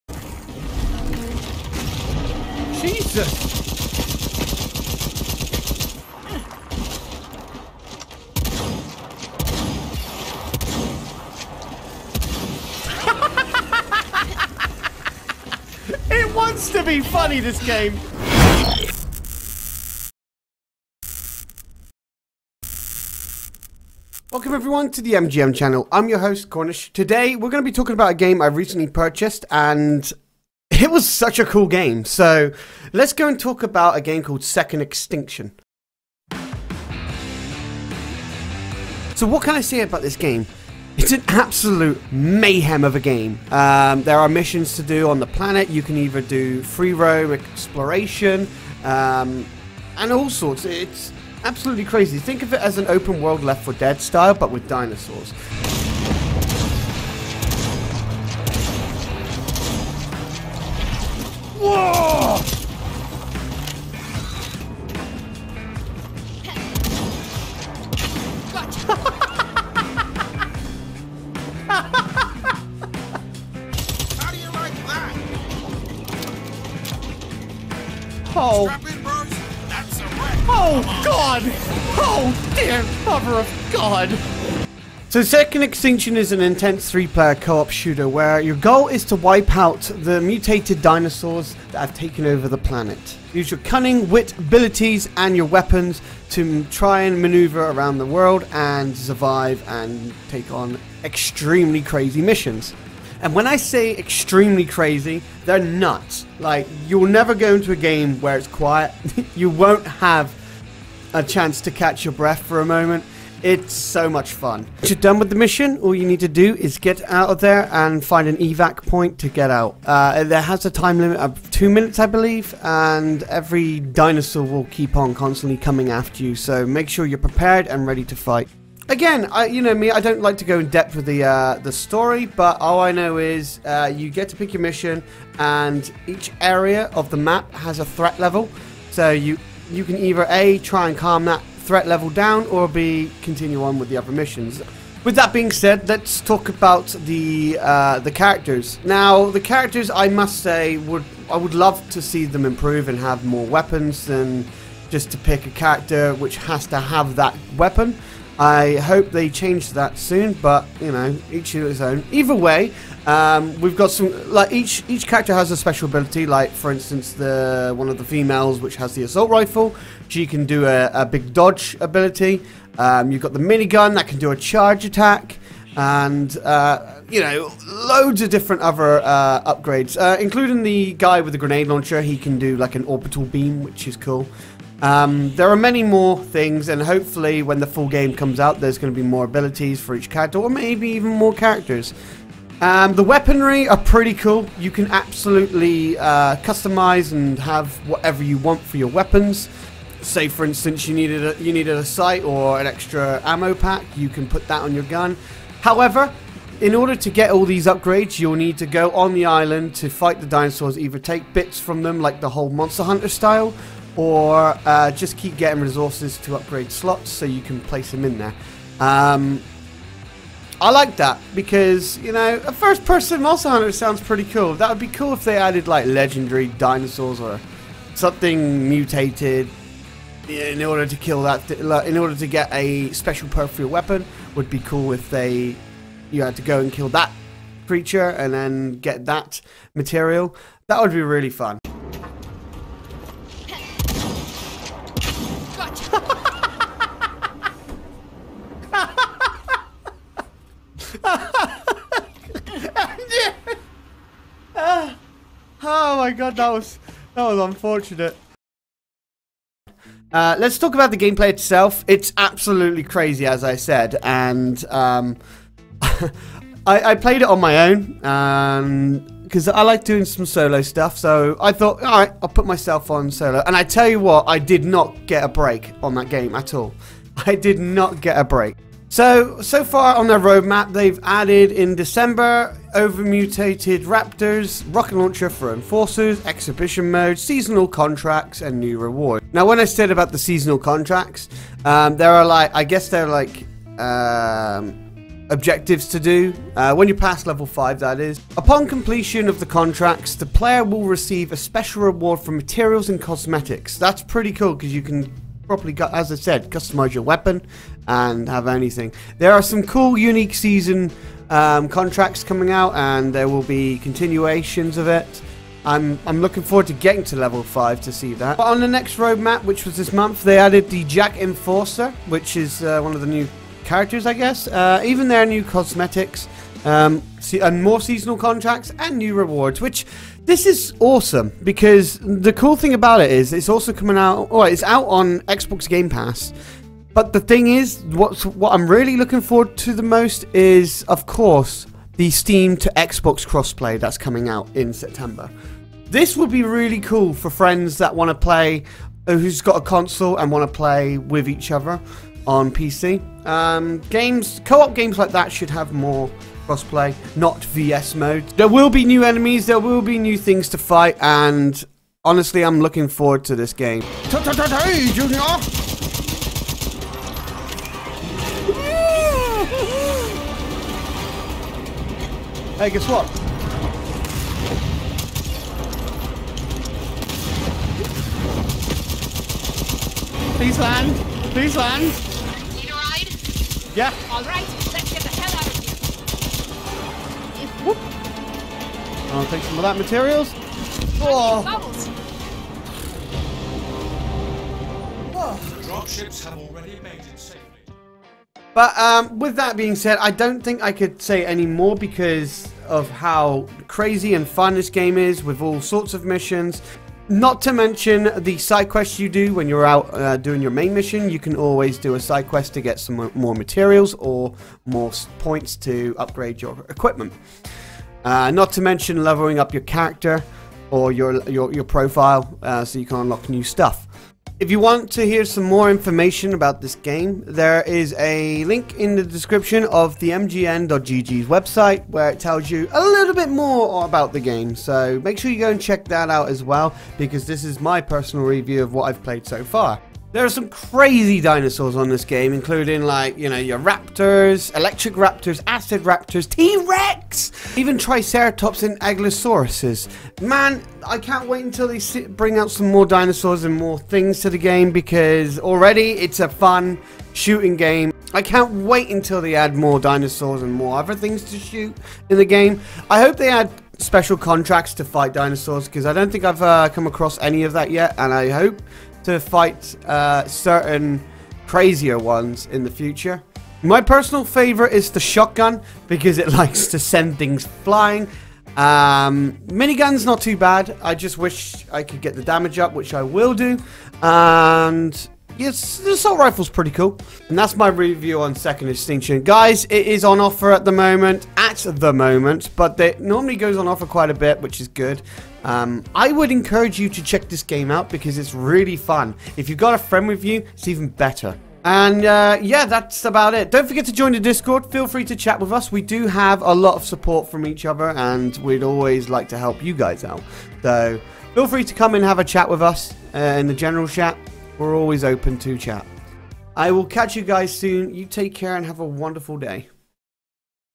Second Extinction - Game Review - MGN TV
Second Extinction is an intense 3 player co-op shooter, where your goal is to wipe out the mutated dinosaurs that have taken over the planet.
With the amount of fun, this game brings this is a great title to pick either with a friend or without.
With lots of content coming to the game this is going to be one of the games you can enjoy and have in your library